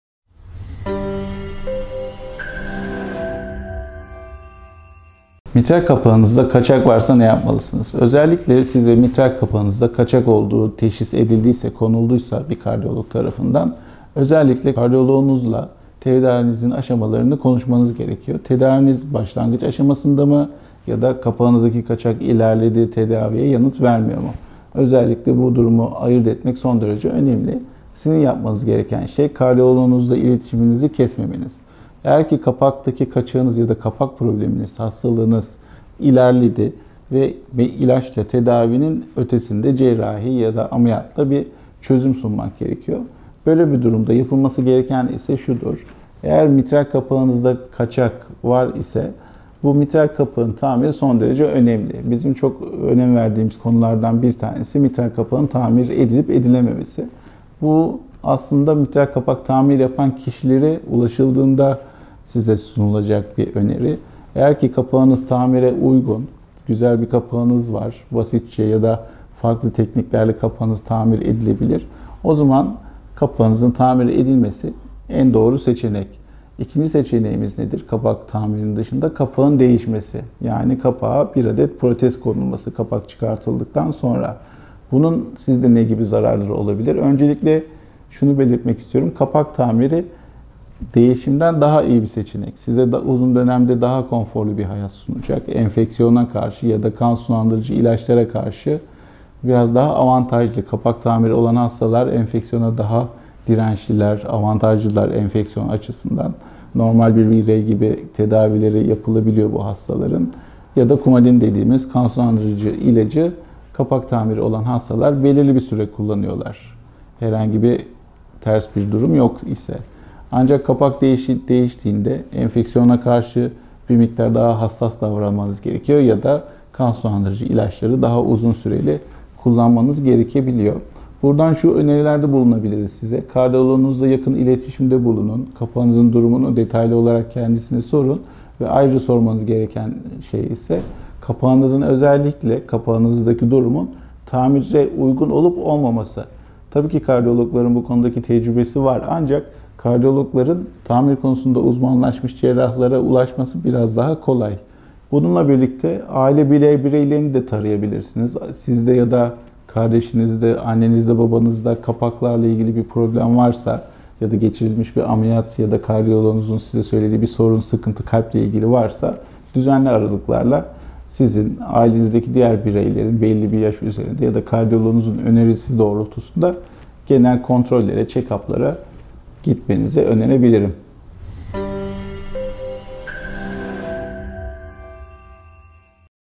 Mitral kapaktaki kaçak tedavisi nasıl yapılır? - Prof. Dr. Burak Onan (Kalp ve Damar Cerrahisi)
Memorial Bahçelievler Hastanesi Kalp ve Damar Cerrahisi Bölümü'nden Prof. Dr. Burak Onan, mitral kapaktaki kaçak tedavisi hakkında bilgi verdi.